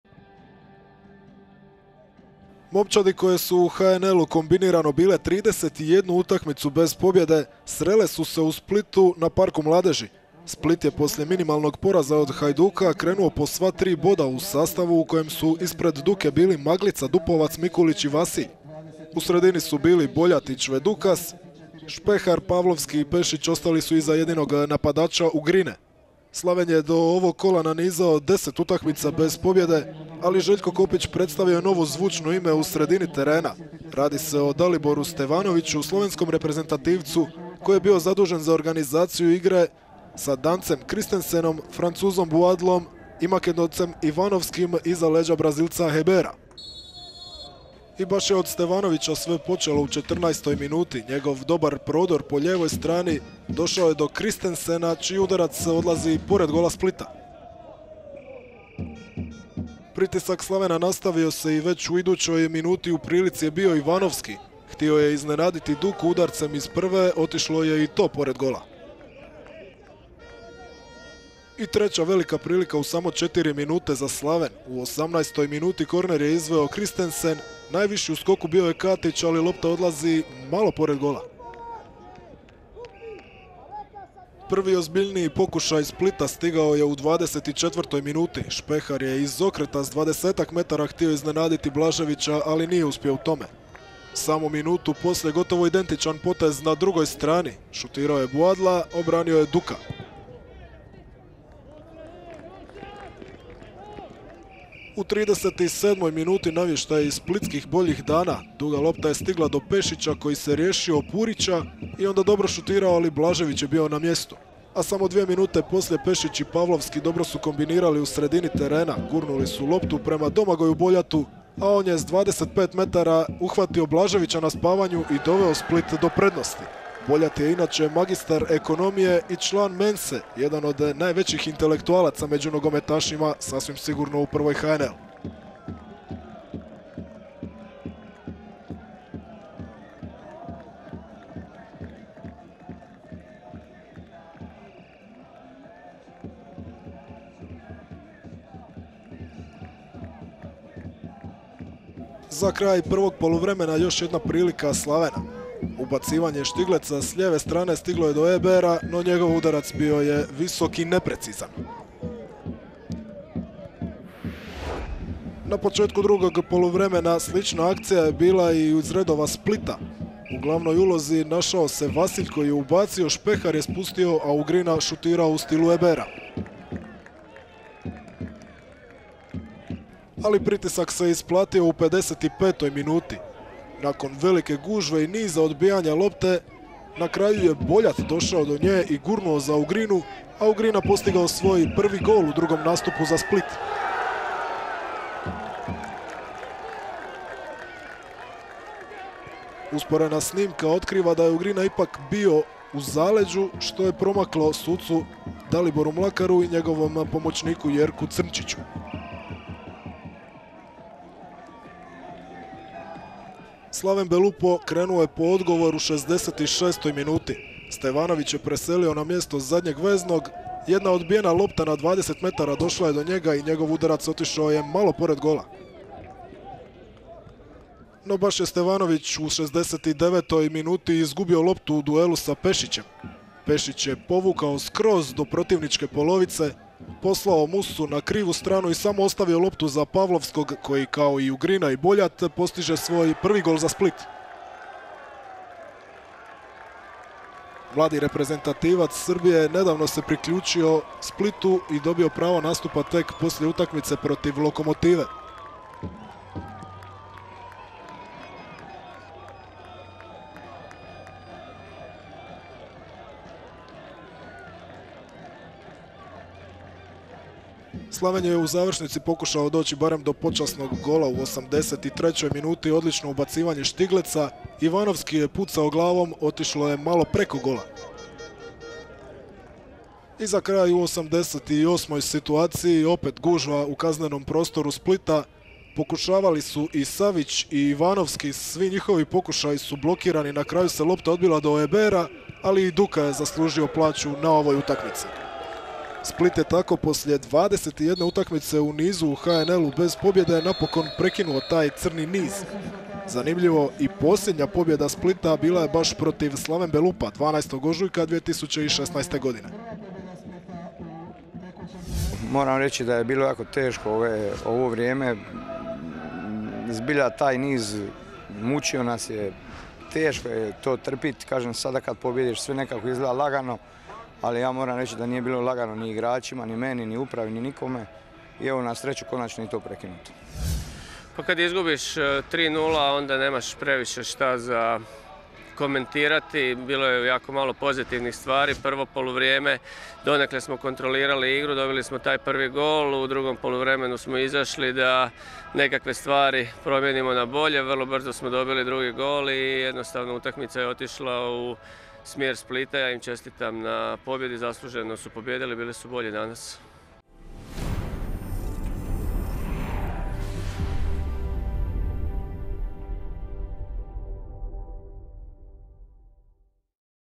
HNTV sažetak: SPLIT vs SLAVEN BELUPO 3:0 (13.kolo, MAXtv Prva liga 16/17)
MAX TV Prva liga: Prva pobjeda Splita u sezoni

U prvoj utakmici 13. kola MAX TV Prve lige nogometaši Splita su na svom stadionu s uvjerljivih 3-0 pobijedili Slaven Belupo.

Bila je to prva pobjeda Splita u aktualnoj sezoni. Bila je to, ujedno, i prva pobjeda Splita poslije čak 21 prvenstvene utakmice, a zadnje je slavlje Split ima baš protiv sastava iz Koprivnice, kada ga je pobijedio tijekom ožujka u prošlom prvenstvu.

Domagoj Boljat, Sandro Ugrina i Marko Pavlovski su bili strijelci u tek trećoj utakmici ove sezone u kojoj je Split postigao gol(ove). Za Slaven je, pak, ovo bila 11 utakmica u nizu bez pobjede u MAX TV Prvoj ligi.

MAX TV Prva liga, 13. kolo: Split - Slaven Belupo 3-0 (Boljat 39, Ugrina 55, Pavlovski 69)